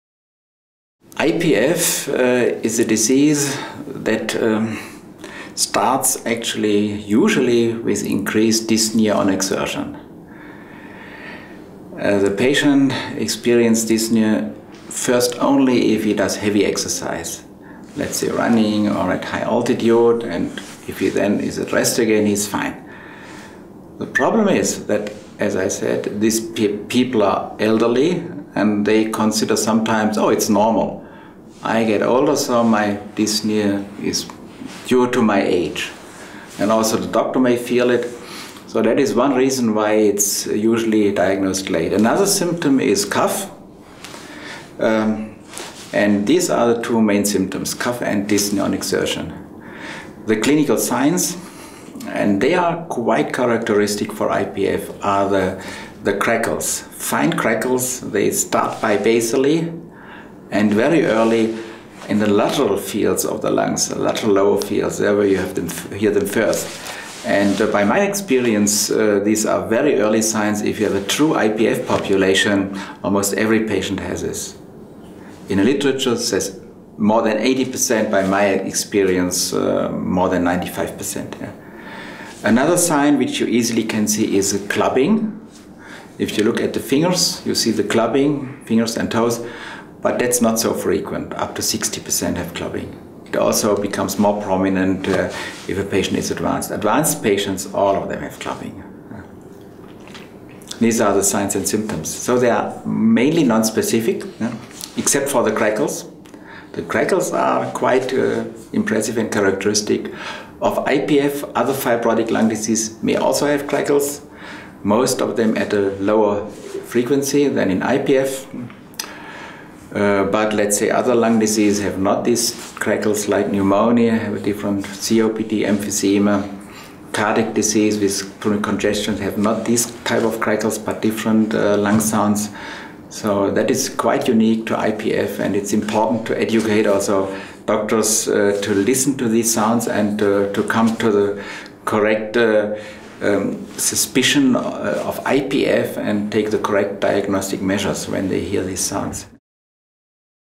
Early signs of Idiopathic Pulmonary Fibrosis (IPF)
Professor Ulrich Costabel discusses the signs and symptoms of idiopathic pulmonary fibrosis (IPF). IPF is a disease which starts with cough and increased dyspnea on exertion. The clinical signs of IPF are bibasilar inspiratory ‘Velcro crackles’, distinctive, Velcro-like crackling sounds that can be detected through a stethoscope.

Healthcare professionals outside the US, UK, and Canada, listen to the example patient auscultations of IPF and other pulmonary diseases,